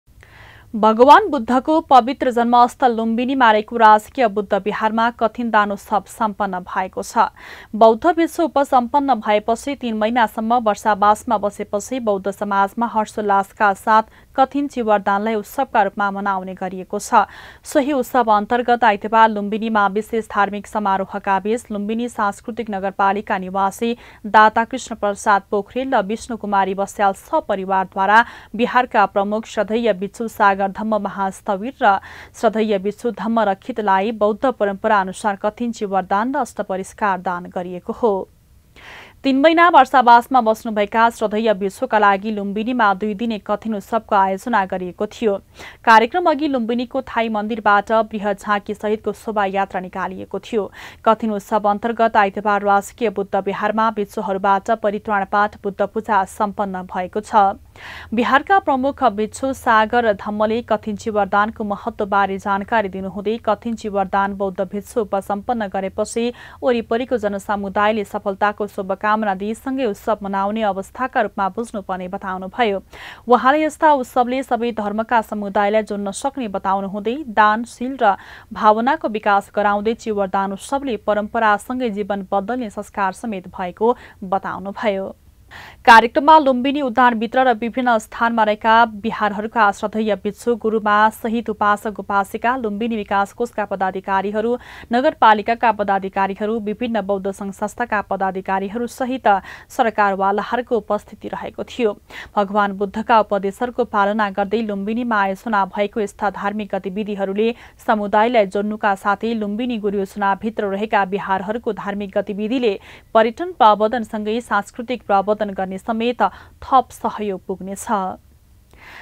राजकिय बुद्ध विहार लुम्बिनीमा कथीन चीवर दान सम्पन्न | BODHI NEWS | BODHI TV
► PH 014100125